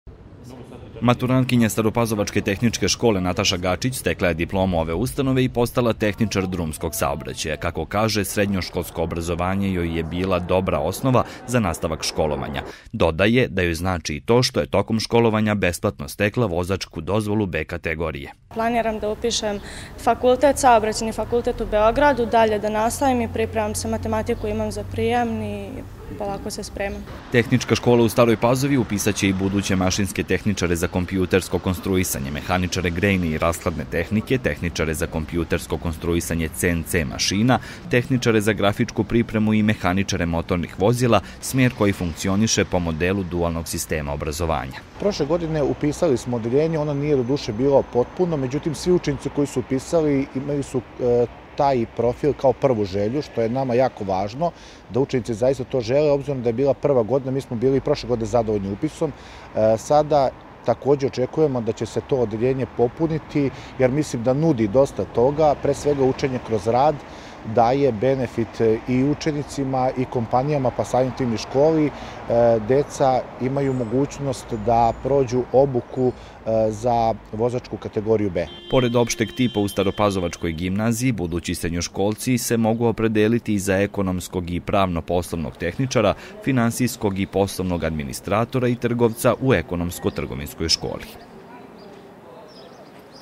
U staropazovačkim srednjim školama 350 slobodnih mesta
350 slobodnih mesta na raspolaganju je budućim srednjoškolcima u 3 srednje škole na teritoriji opštine Stara Pazova, a pored toga Škola za osnovno i srednje obrazovanje „Anton Skala“ u kojoj se obrazuju deca sa smetnjama u razvoju upisaće po šestoro bravara i ručnih tkača.